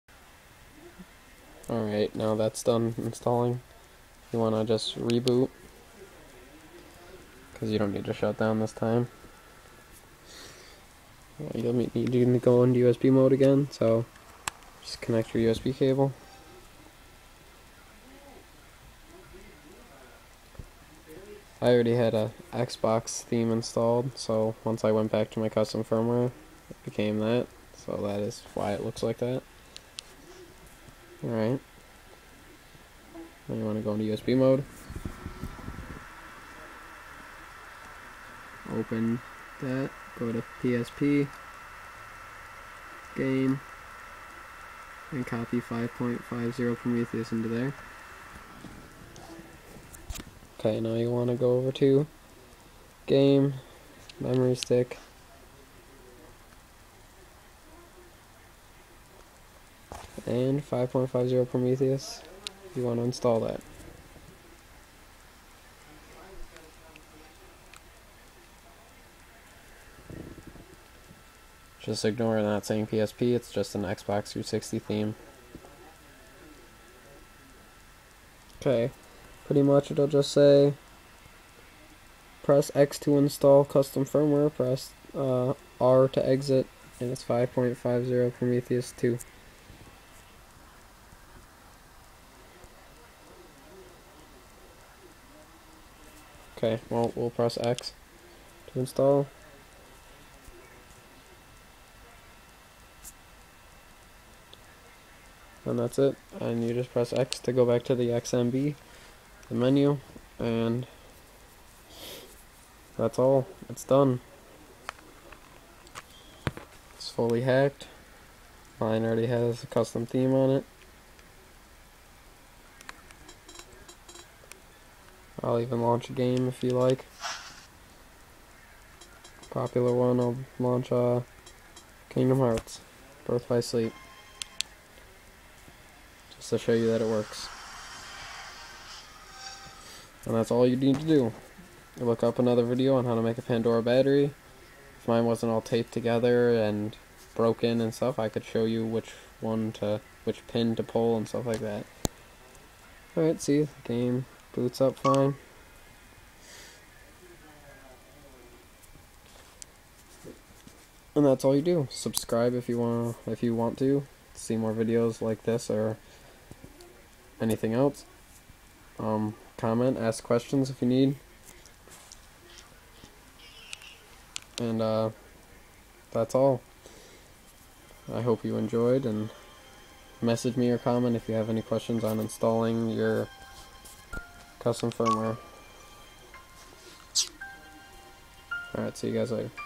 Part 7 - How to Mod PSP 1000/2000 Pandora Battery MMS 6.31 Hack
I show you how to install Custom Firmware on any PSP 1000 and many PSP 2000's using a Pandora's Battery and a Magic Memory Stick.

PART 2 -
PART 3 -
PART 4 -
PART 5 -
PART 6 -

Sorry for so many parts, Movie Maker doesn't support MP4 files so I'm shit outta luck. I need to make my future video in one take.